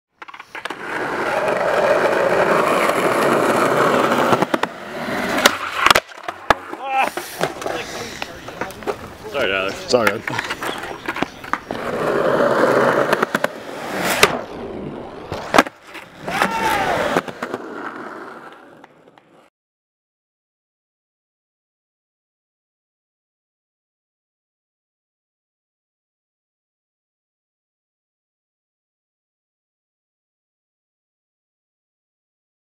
Cody Lessard: Big Heel
Cody Lessard doing I like a champ on the Derry Skateparks Bank.

Follow me @Tyler_Krupa for updates on when new videos will be posted and other stuff like that.

Filmed with:
Canon EOS Rebel T3i
Opteka 6.5mm Ultra Fisheye Lens
F&V Z96 on camera LED light

ThrasherMagazine Double Rock: Bones Ams